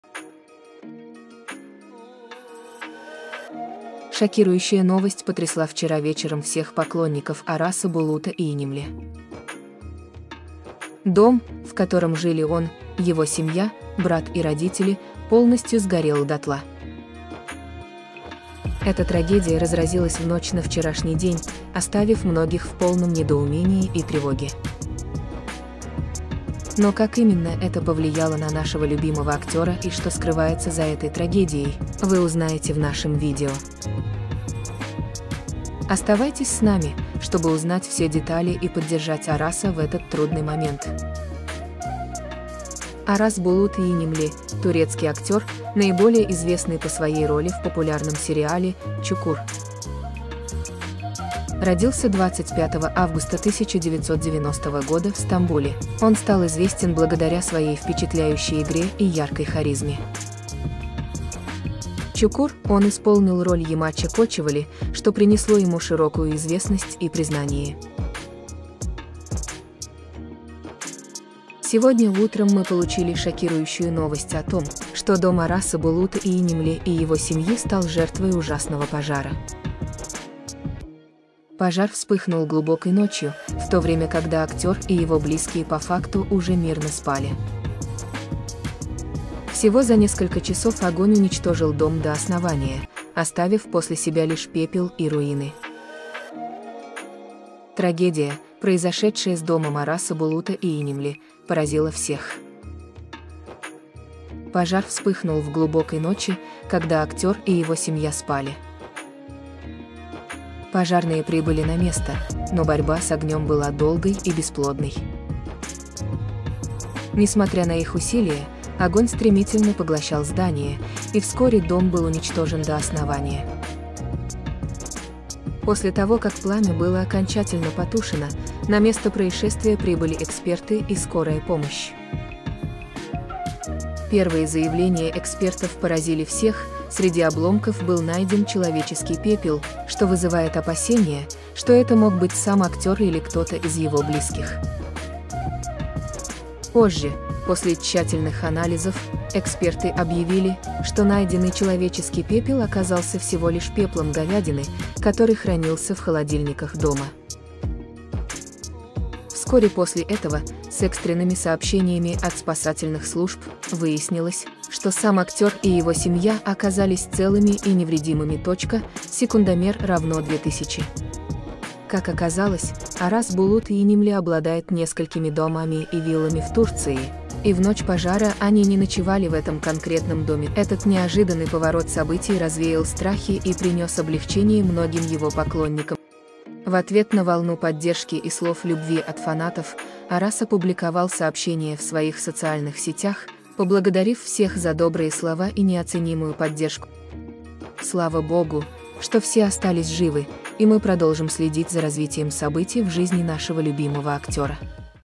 Найдено тело Араса Булута Ийнемли после пожара его дома
Шокирующая новость потрясла вчера вечером всех поклонников Араса Булута Ийнемли. Дом, в котором жили он, его семья, брат и родители, полностью сгорел дотла. Эта трагедия разразилась в ночь на вчерашний день, оставив многих в полном недоумении и тревоге. Но как именно это повлияло на нашего любимого актера и что скрывается за этой трагедией, вы узнаете в нашем видео. Оставайтесь с нами, чтобы узнать все детали и поддержать Араса в этот трудный момент.